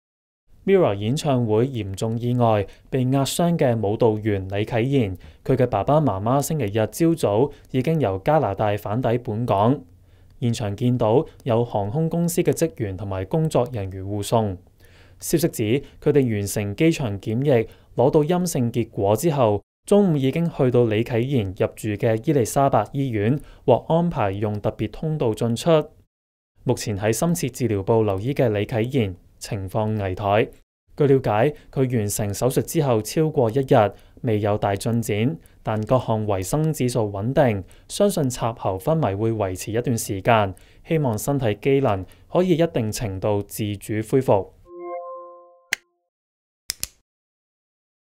MIRROR演唱會｜阿Mo術後未見大進展料仍會昏迷 父母已到院探望
MIRROR演唱會第四場發生嚴重事故，一部大型屏幕墮下，壓傷兩名舞蹈員，其中李啟言（阿Mo）於伊利沙伯醫院留醫深切治療部留醫，並進行了兩次手術，情況危殆。